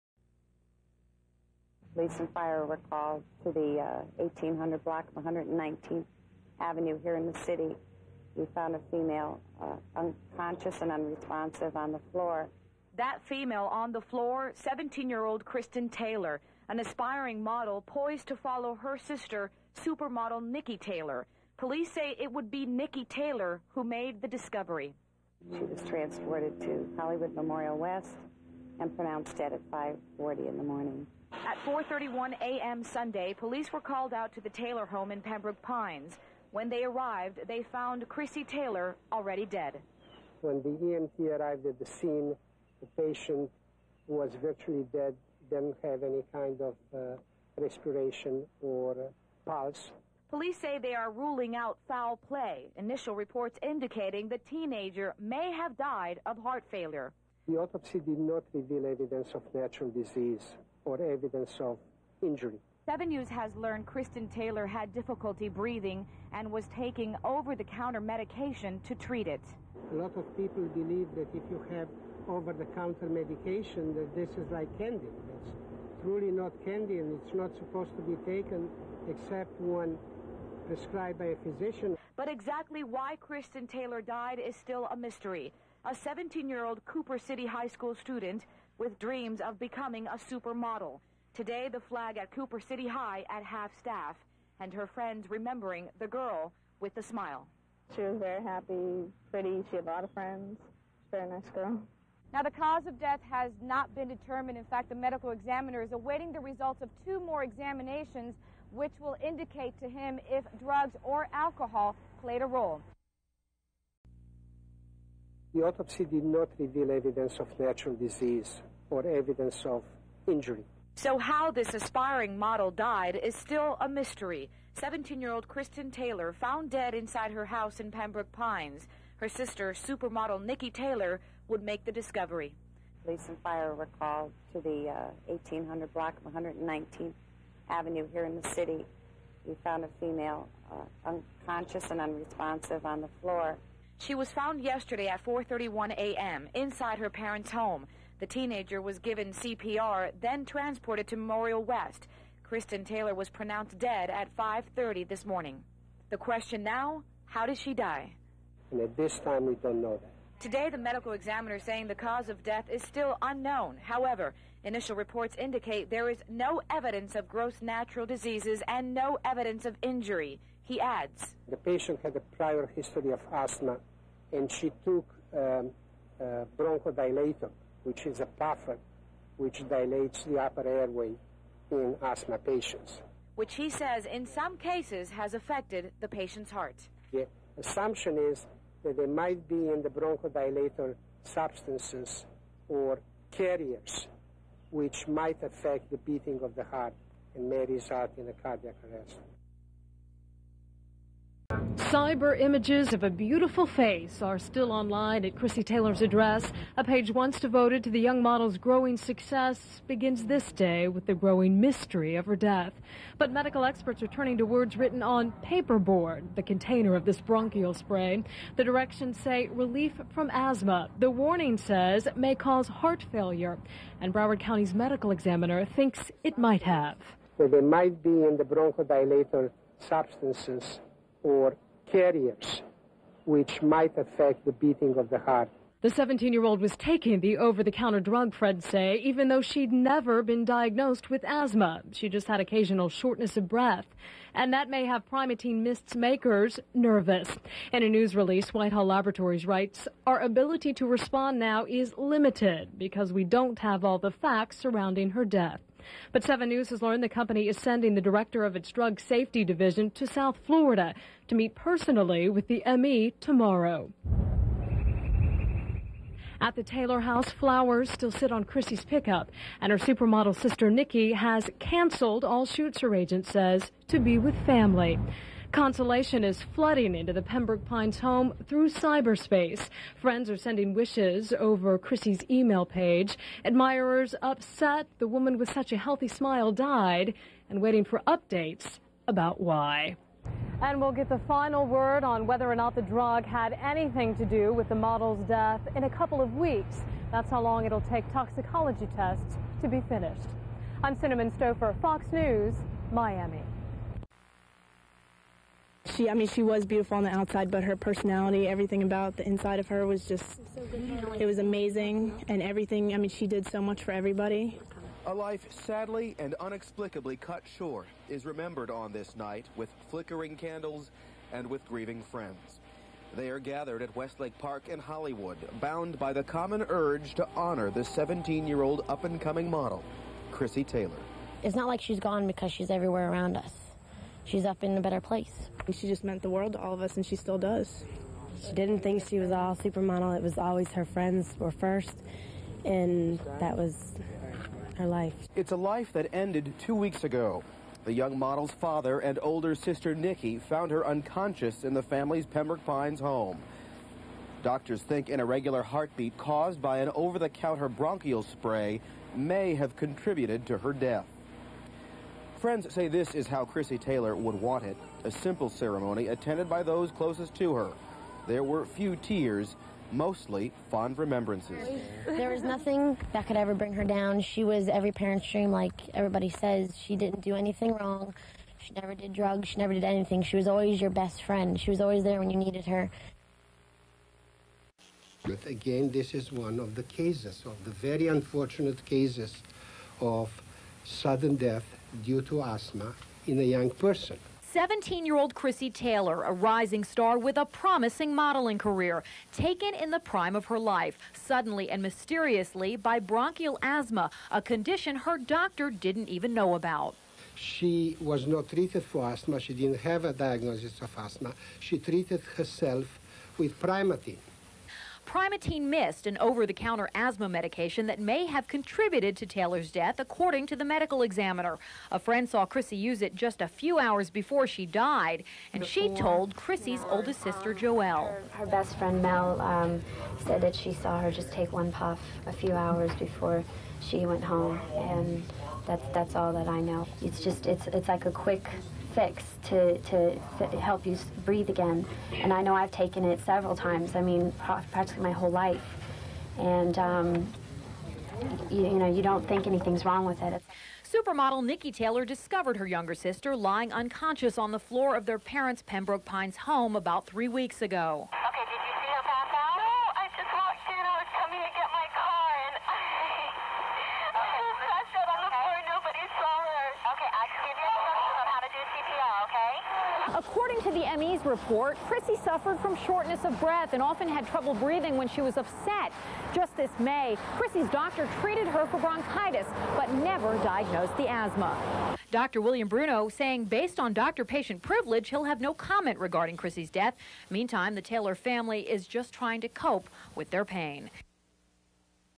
Krissy Taylor , Fox News 1995
On July 2, 1995, Niki Taylor found Krissy lying unconscious on the floor of their family's home in Florida. After attempts to revive Taylor by her family and authorities failed, she was rushed to Memorial Hospital West and pronounced dead at 5:39 a.m. She was 17 years old.

Taylor was known to have had allergies that congested her upper respiratory system.

The official cause of death was an acute asthma attack complicated by sudden cardiac arrhythmia. However, she had no prior history of asthma, and no symptoms of a heart condition. Uncertain of the medical examiner's diagnosis, the Taylor family hired independent experts to study tissue samples of Taylor's heart muscle, and concluded that the more likely cause of her death was a rare cardiac disease called arrhythmogenic right ventricular dysplasia.

Taylor’s mother, Barbara, is now involved with the Cardiac Arrythmias Research and Education (CARE) Foundation